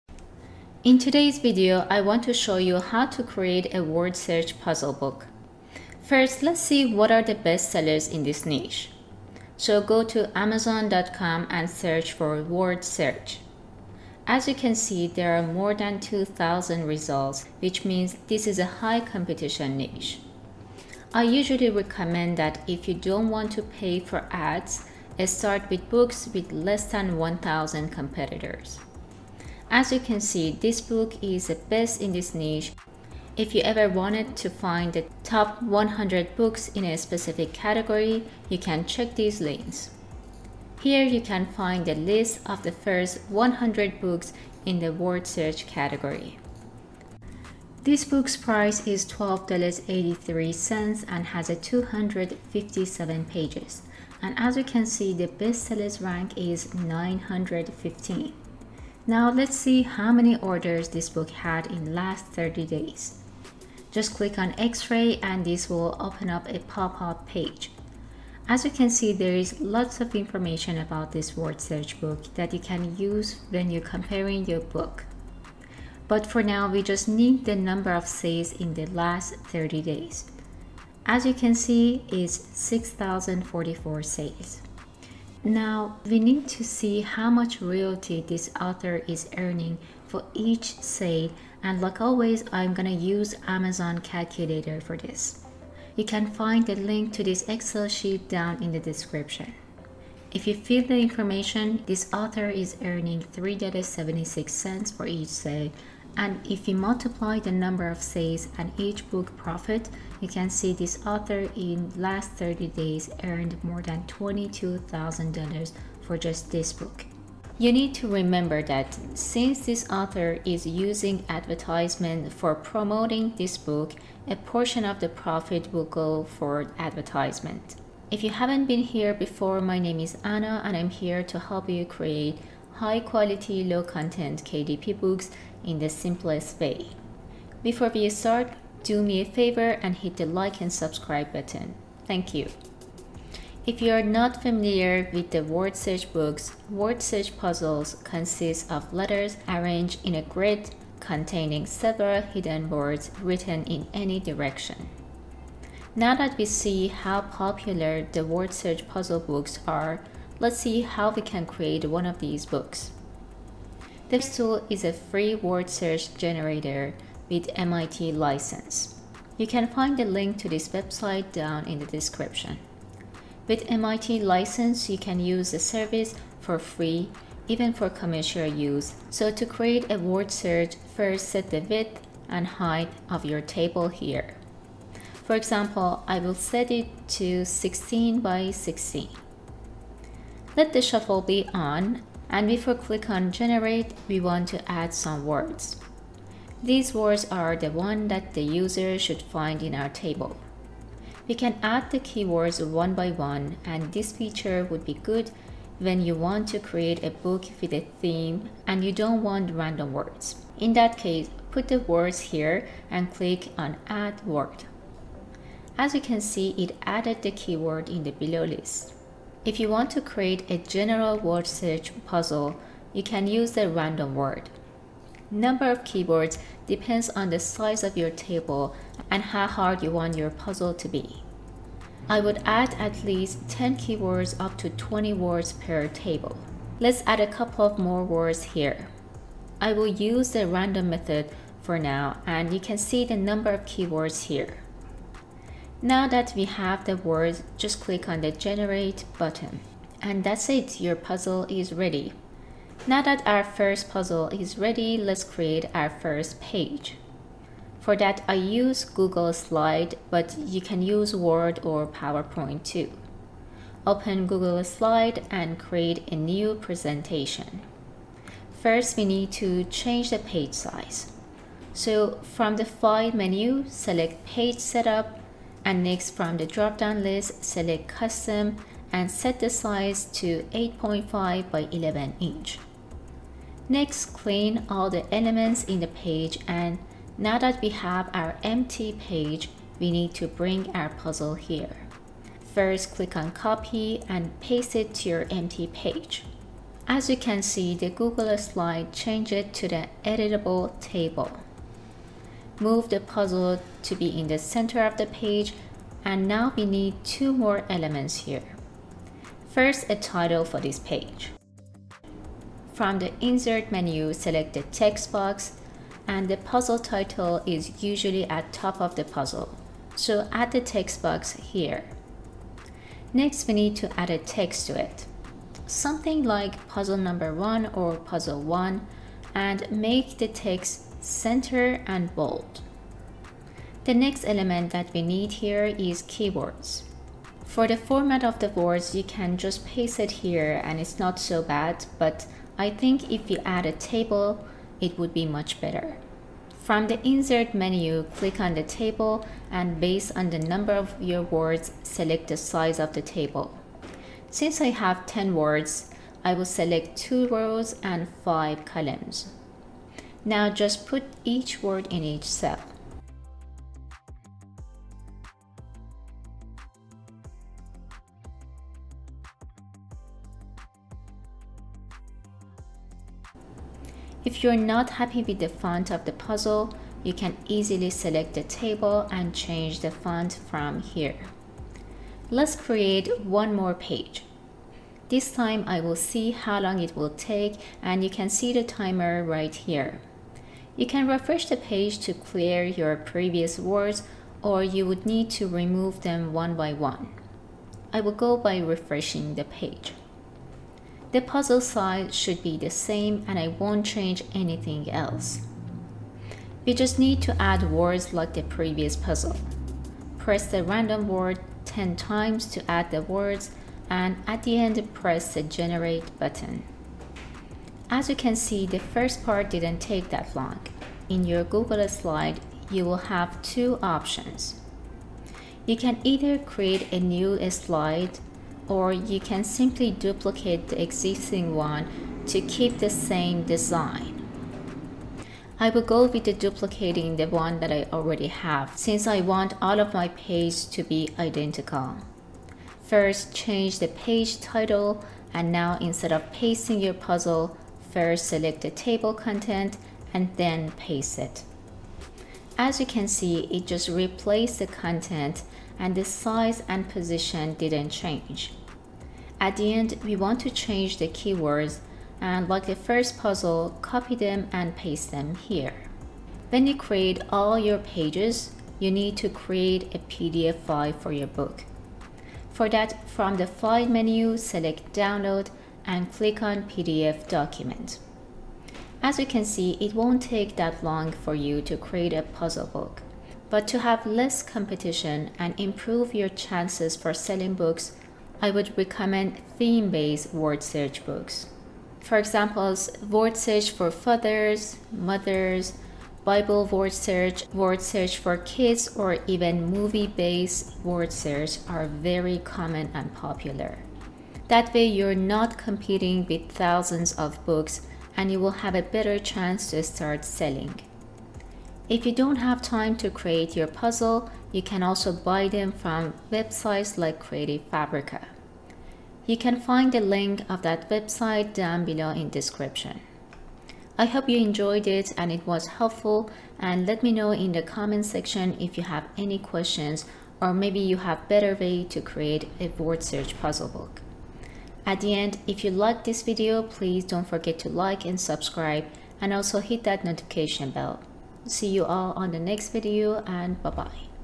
How to Make Word Search Book for Amazon KDP With Free Software and Make 22000$ Per Month
In today's video, I want to show you how to create a word search book. tools I’m going to introduce in this video can be used for any type of word search book. So get ready and follow along to create a word search puzzle in just a few minutes.
I'm going to show you how to create an word search book using free online software. They make the perfect gift for Christmas, and birthday presents.

📌Helpful websites:
Drawing tablet